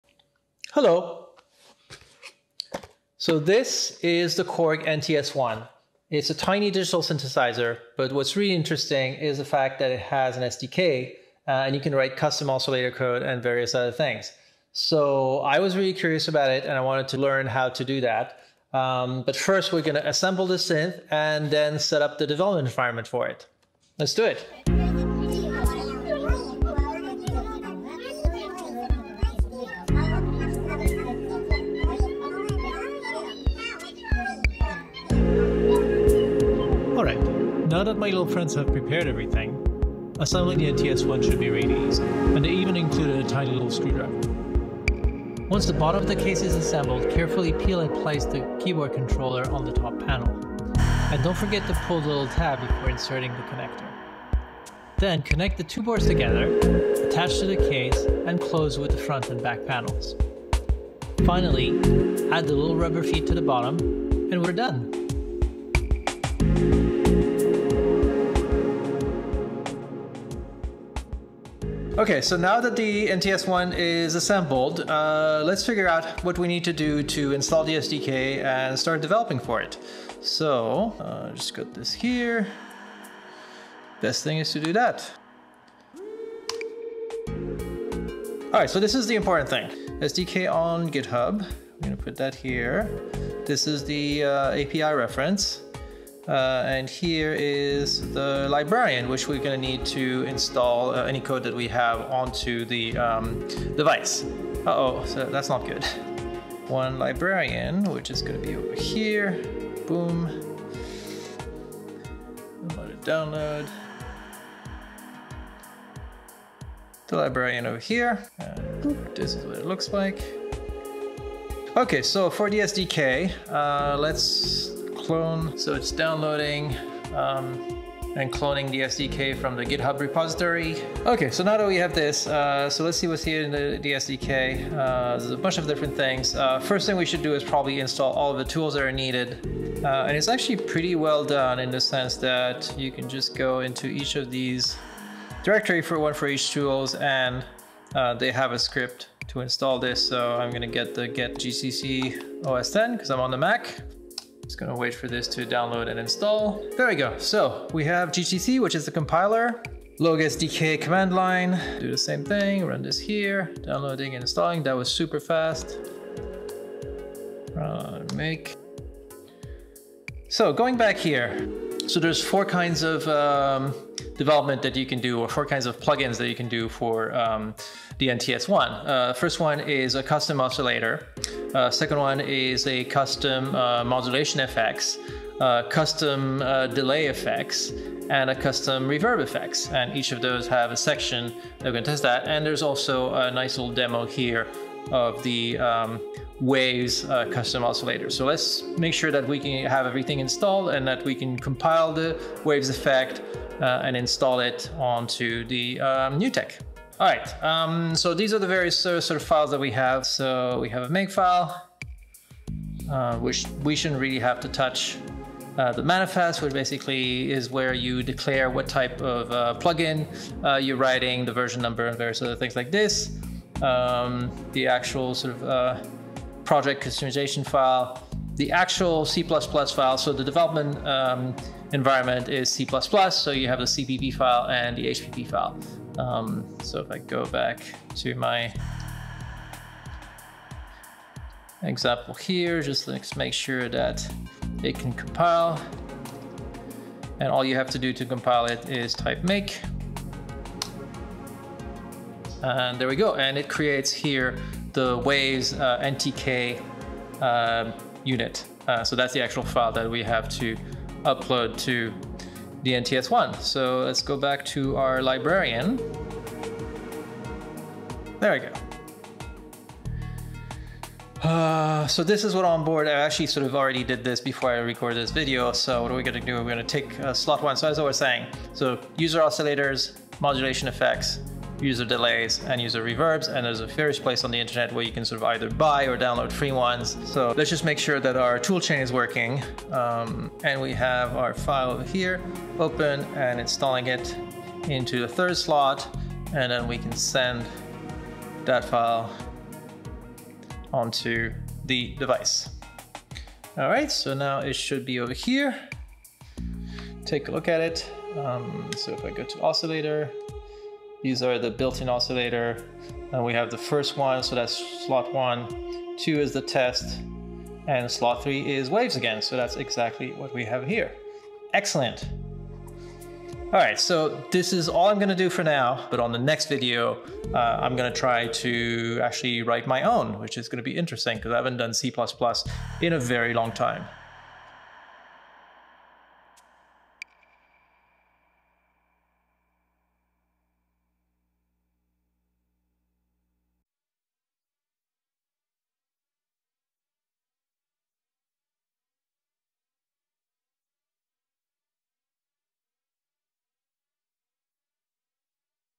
Hello World: programming the Korg NTS-1 (part 1)
I just got the Korg NTS-1 for my first Quarantine Birthday, and while it sounds really good for a tiny digital synth, I'm really interested in the SDK and learning how to write custom Oscillators and Effects for the Korg 'logue platform.

In the first video in this series, I'm assembling the little flat-pack synth, ( it even comes with a tiny tool, just like IKEA furnitures ) and setting up the development environment on my MacBook Pro.

PART 2 is Ready!